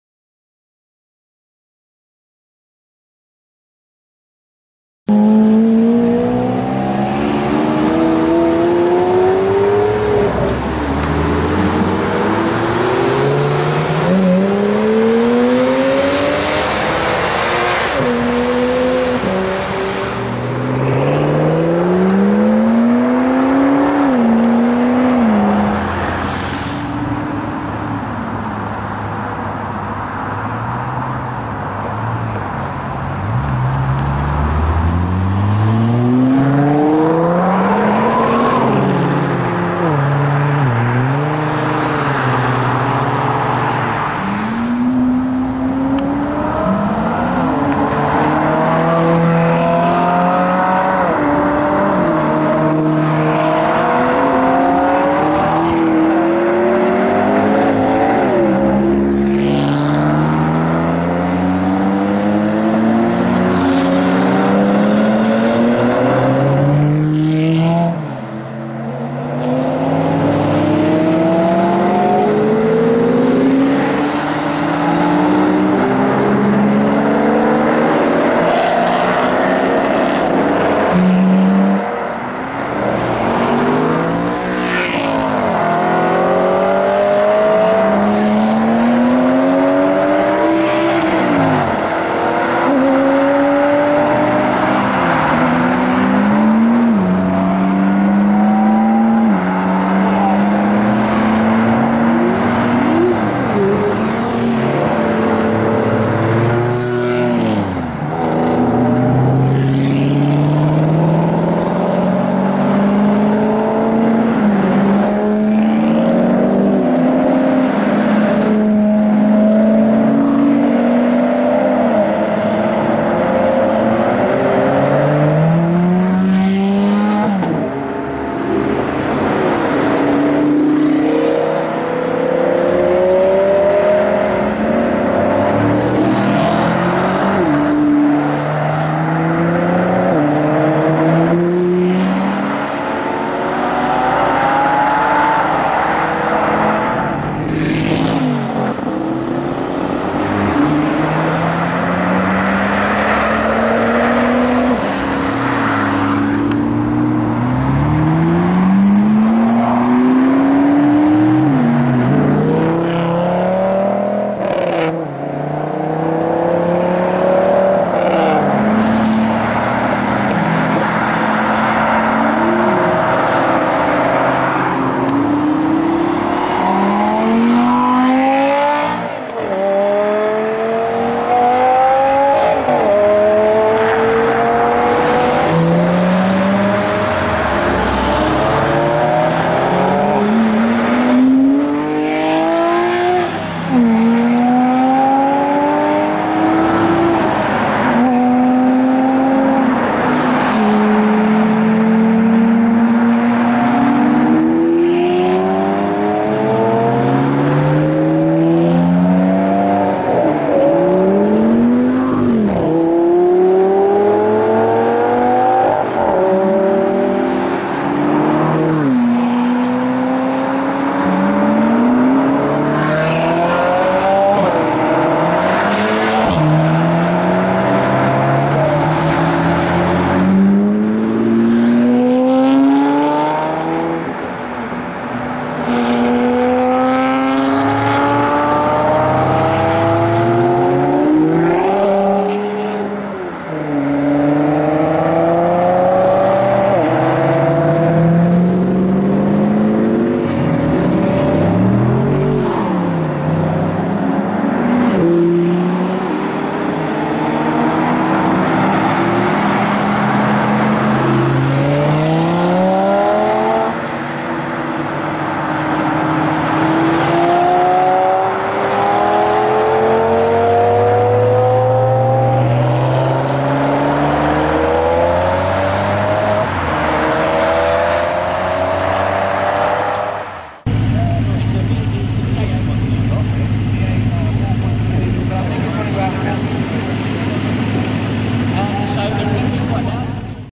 WET & FAST SUPERCARS: Motorway Blast!
Amazing Supercar Sounds as they blast onto the UK motorway in the wet! LOUD Supercar engines!